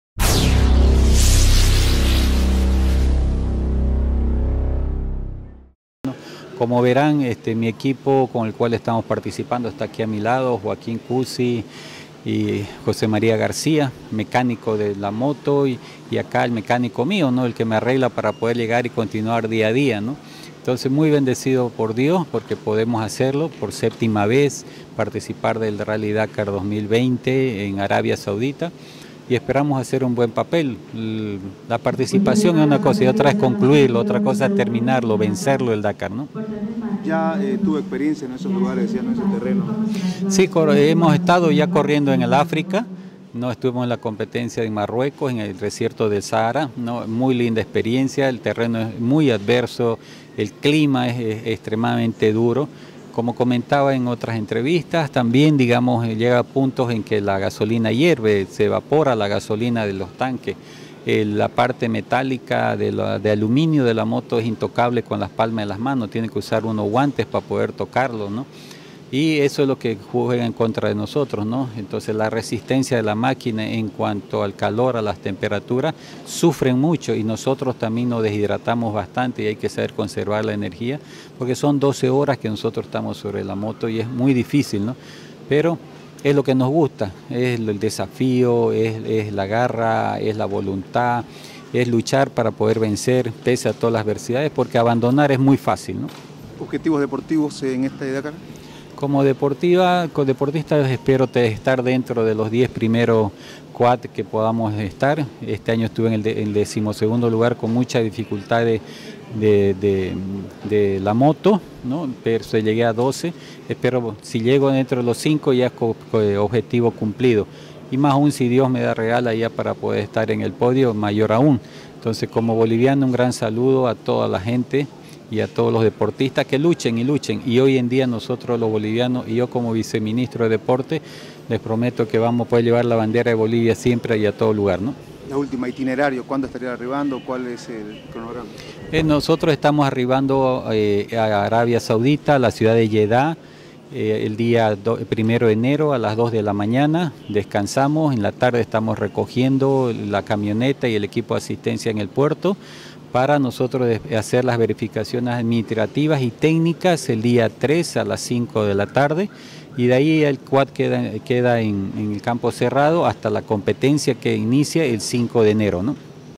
Leo Martínez
Leonardo Martínez, único boliviano en la categoría cuadriciclos en el Dakar 2020